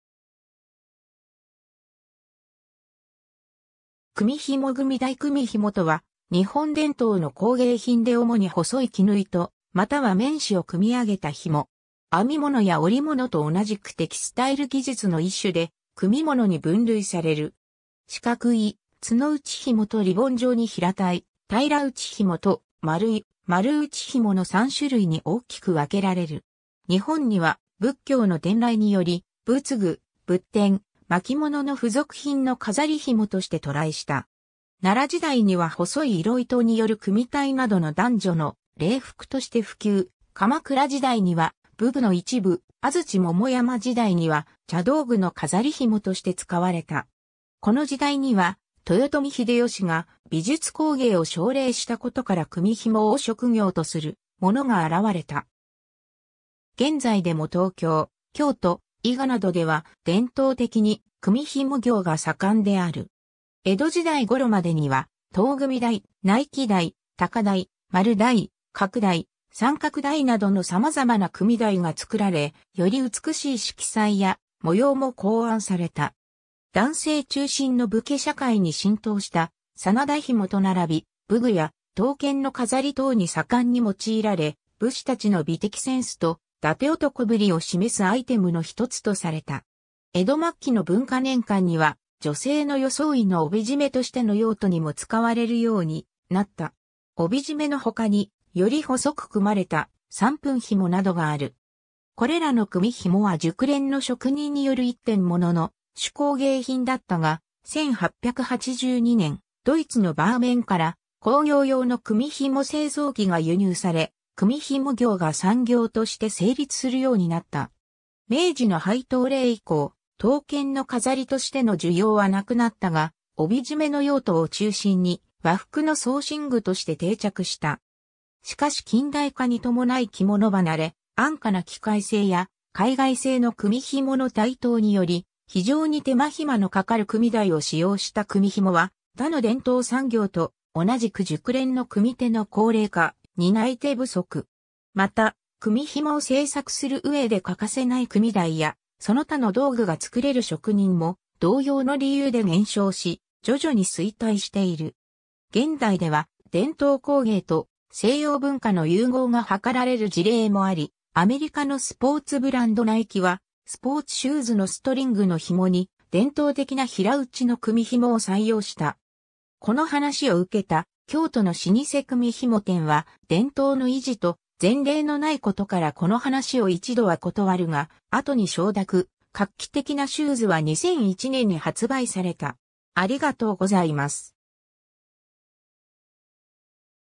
組み紐
組み紐（くみひも） 組台 組紐（くみひも）とは、日本伝統の工芸品で主に細い絹糸、または綿糸を組み上げた紐。
 編み物や織物と同じくテキスタイル技術の一種で組物に分類される。
四角い「角打ち紐」とリボン状に平たい「平打紐」と、丸い「丸打紐」の3種類に大きく分けられる。
 日本には仏教の伝来により、仏具、仏典、巻物の付属品の飾り紐として渡来した。
奈良時代には細い色糸による組み帯などの男女の礼服として普及、鎌倉時代には武具の一部、安土桃山時代には茶道具の飾り紐として使われた。
この時代には、豊臣秀吉が美術工芸を奨励したことから組み紐を職業とする者が現れた。
現在でも東京、京都、伊賀などでは伝統的に、組み紐業が盛んである。
 江戸時代頃までには唐組台、内規台、高台、丸台、角台、三角台などの様々な組台が作られ、より美しい色彩や模様も考案された。
男性中心の武家社会に浸透した「真田紐」と並び、武具や刀剣の飾り等に盛んに用いられ、武士達の美的センスと伊達男ぶりを示すアイテムのひとつとされた。
 江戸末期の文化年間には女性の装いの帯締めとしての用途にも使われるようになった。
帯締めの他に、より細く組まれた「三分紐」などがある。
 これらの組み紐は熟練の職人による一点ものの手工芸品だったが、1882年、ドイツのバーメンから、工業用の組み紐製造機が輸入され、組み紐業が産業として成立するようになった。
明治の廃刀令以降、刀剣の飾りとしての需要はなくなったが、帯締めの用途を中心に和服の装身具として定着した。
 しかし近代化に伴い着物離れ、安価な機械製や海外製の組紐の台頭により、非常に手間暇のかかる組台を使用した組み紐は他の伝統産業と同じく熟練の組み手の高齢化、担い手不足。
また組み紐を制作する上で欠かせない組台やその他の道具が作れる職人も同様の理由で減少し、徐々に衰退している。
 現代では伝統工芸と西洋文化の融合が図られる事例もあり、アメリカのスポーツブランドナイキはスポーツ・シューズのストリングの紐に伝統的な平打ちの組み紐を採用した。
この話を受けた京都の老舗 組み紐店は、伝統の維持と、前例のない事からこの話を一度は断るが、後に承諾、画期的なシューズは2001年に発売された。